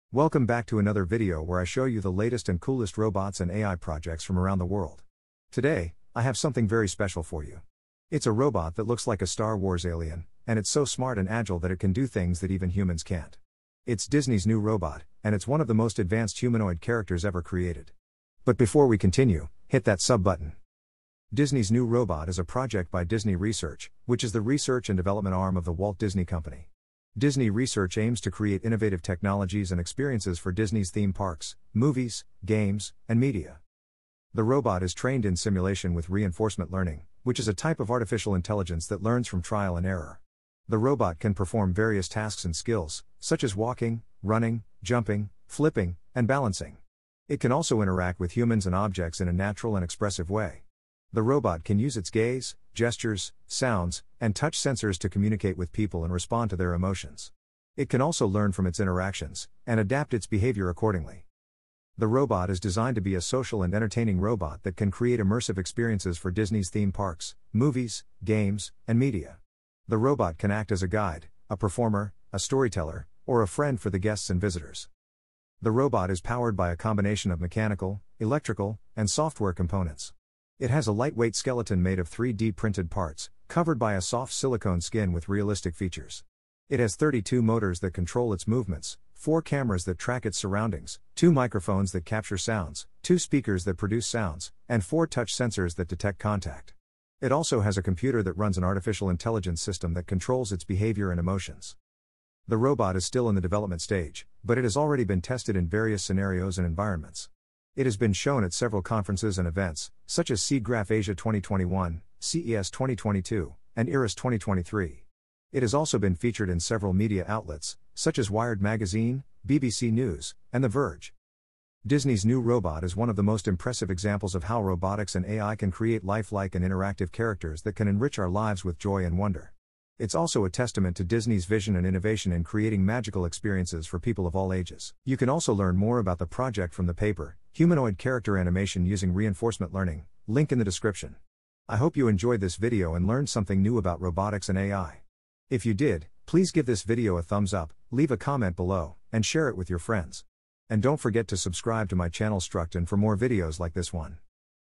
AI Robot From Disney's Star Wars Universe Showcased At IROSS2023
Hey, Structon fans! Welcome back to another video where I show you the latest and coolest robots and AI projects from around the world. Today, I have something very special for you. It's a robot that looks like a Star Wars alien, and it's so smart and agile that it can do things that even humans can't. It's Disney's new robot, and it's one of the most advanced humanoid characters ever created.

Disney's new robot is a project by Disney Research, which is the research and development arm of The Walt Disney Company. Disney Research aims to create innovative technologies and experiences for Disney's theme parks, movies, games, and media. Disney Research has been working on humanoid robots for a long time, and they have created some amazing characters, such as the Na'vi Shaman from Avatar, the Lumiere from Beauty and the Beast, and the Baby Groot from Guardians of the Galaxy.

But Disney's new robot is different from these characters because it's not based on any existing character or franchise. It's a completely original creation, inspired by the Star Wars universe. The robot resembles an alien species that has never been seen before in Star Wars movies or shows. The robot has a unique appearance, with green skin, yellow eyes, horns, and spikes. The robot also has a distinctive personality, with a sense of humor, curiosity, and adventure.

The robot is trained in simulation with reinforcement learning, which is a type of artificial intelligence that learns from trial and error. The robot can perform various tasks and skills, such as walking, running, jumping, flipping, and balancing. It can also interact with humans and objects in a natural and expressive way. The robot can use its gaze, gestures, sounds, and touch sensors to communicate with people and respond to their emotions. It can also learn from its interactions, and adapt its behavior accordingly.

The robot is designed to be a social and entertaining robot that can create immersive experiences for Disney's theme parks, movies, games, and media. The robot can act as a guide, a performer, a storyteller, or a friend for the guests and visitors. The robot can also participate in various activities and events, such as parades, shows, games, and contests.

The robot is powered by a combination of mechanical, electrical, and software components. It has a lightweight skeleton made of 3D-printed parts, covered by a soft silicone skin with realistic features. It has 32 motors that control its movements, 4 cameras that track its surroundings, 2 microphones that capture sounds, 2 speakers that produce sounds, and 4 touch sensors that detect contact. It also has a computer that runs an artificial intelligence system that controls its behavior and emotions.

The robot is still in development but has already been tested in various scenarios and environments. It has been shown at several conferences and events, such as SIGGRAPH Asia 2021, CES 2022 , and IROS2023. It has also been featured in several media outlets , such as Wired Magazine, BBC News, and The Verge.

Disney's new robot is one of the most impressive examples of how robotics and AI can create lifelike and interactive characters that can enrich our lives with joy and wonder. It's also a testament to Disney's vision and innovation in creating magical experiences for people of all ages.

If you want to see Disney's new robot in action at IROS2023 ⁴, you can watch this video: [Disney's New Robot at IROS2023]. You can also learn more about the project from this paper
I hope you enjoyed this video and learned something new about robotics and AI.